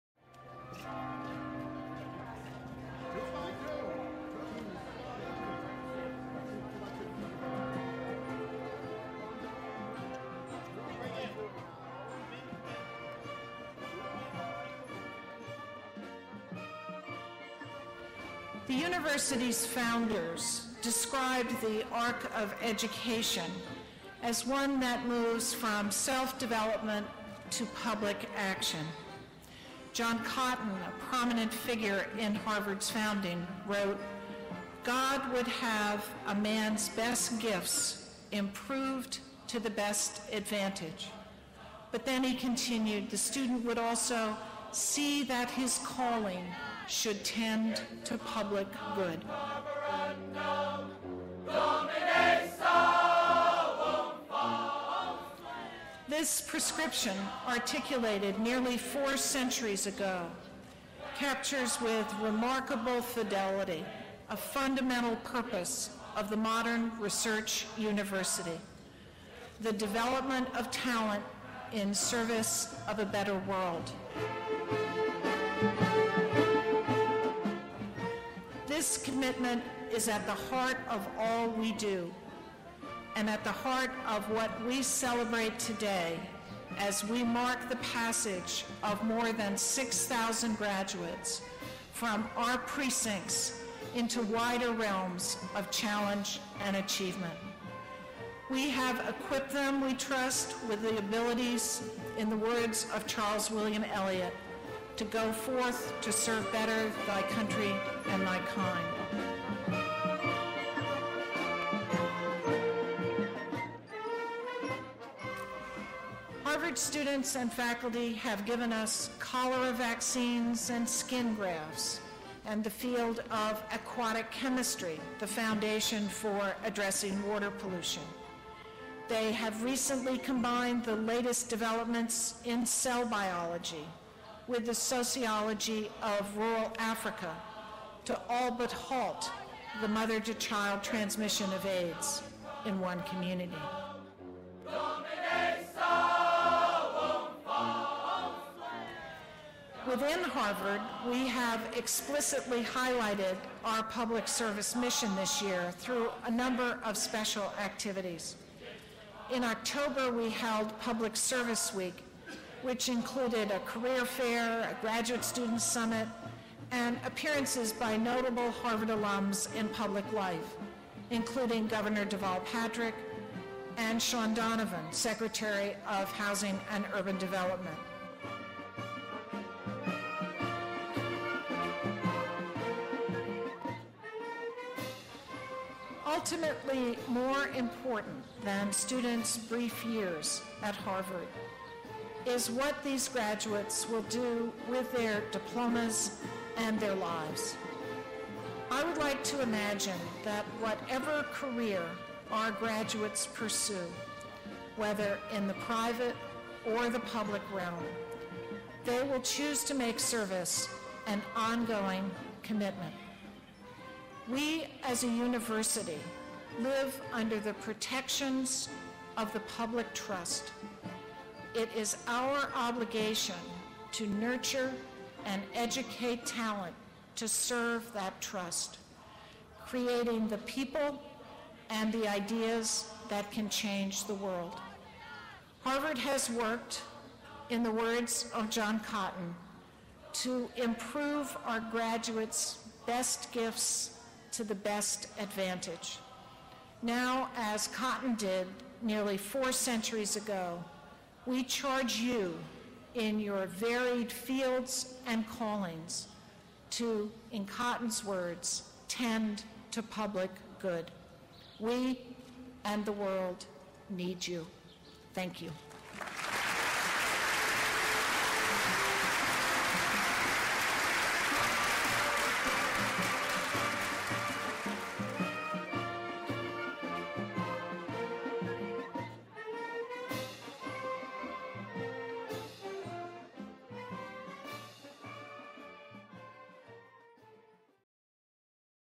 Harvard University: Year in Pictures 2009-2010
Experience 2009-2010 at Harvard University through photographs.